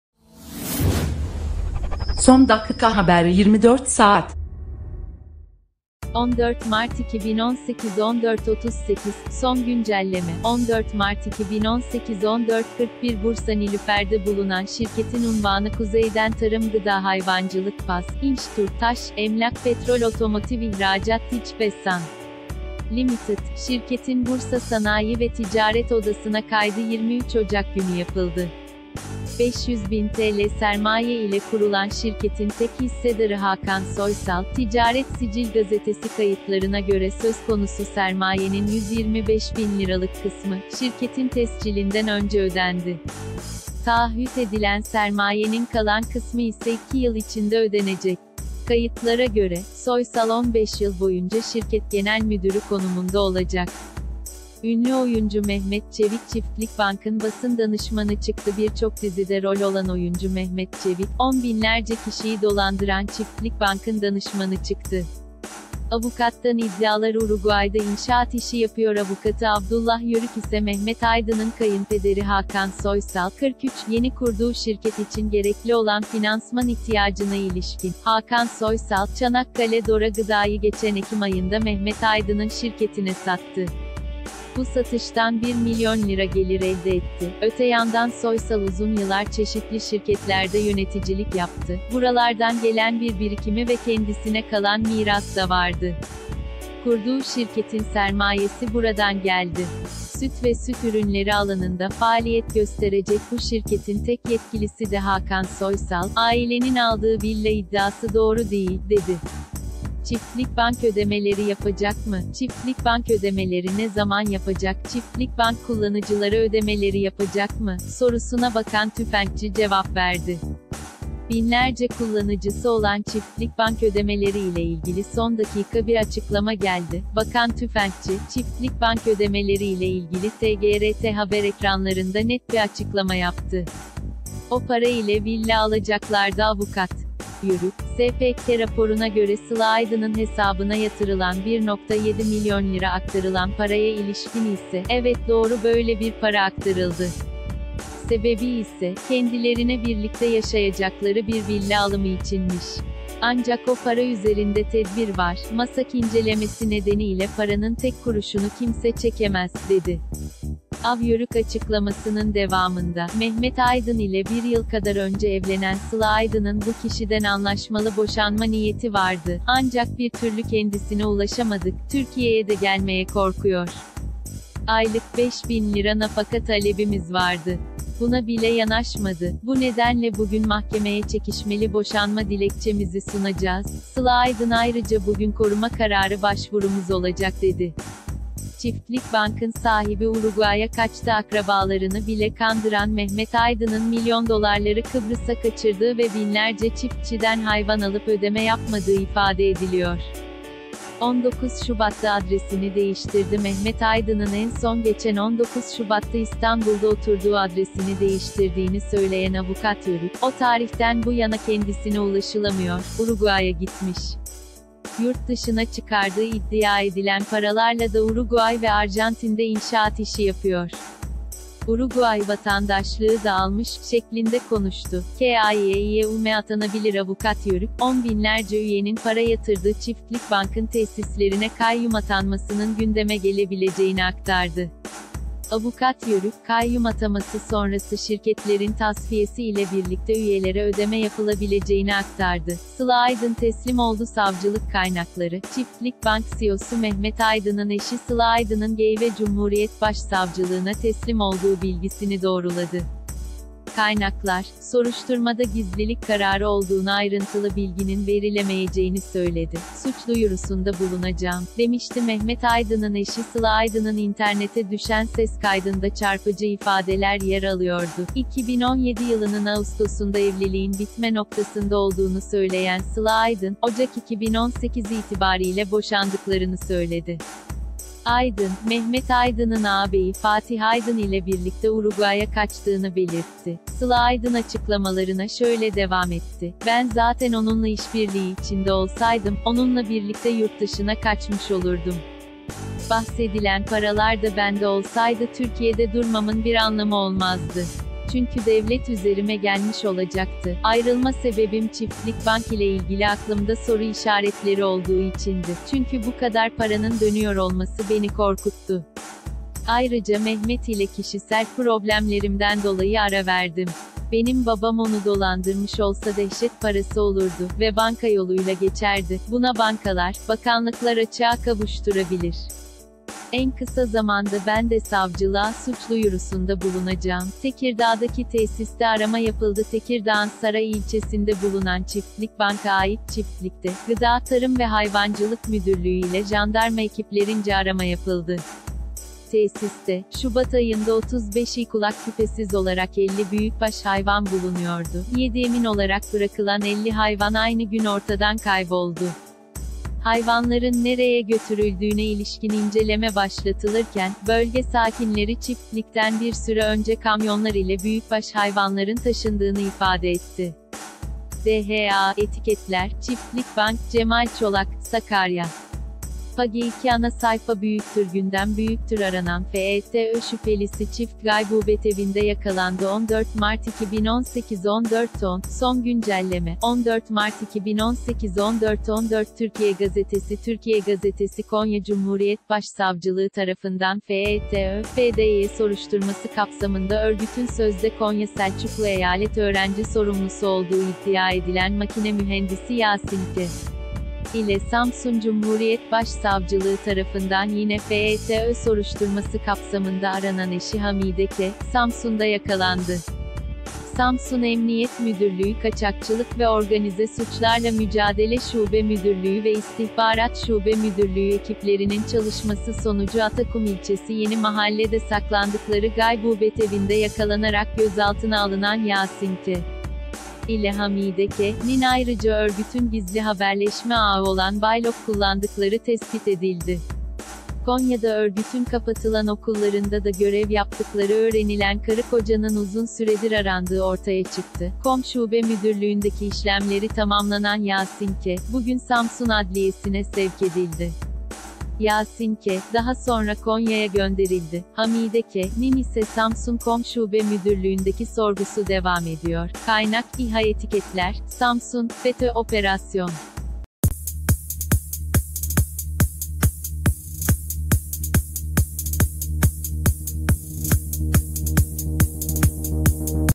Çiftlik Bank şirket sahibi Mehmet Aydın'ın eşi teslim oldu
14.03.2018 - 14:38 | Son Güncelleme: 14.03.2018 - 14:41Bursa Nilüfer’de bulunan şirketin unvanı Kuzeyden Tarım Gıda Hayvancılık Paz. İnş. Tur. Taş. Emlak Petrol Otomotiv İhracat Tic. Ve San. Limited… Şirketin Bursa Sanayi ve Ticaret Odası’na kaydı 23...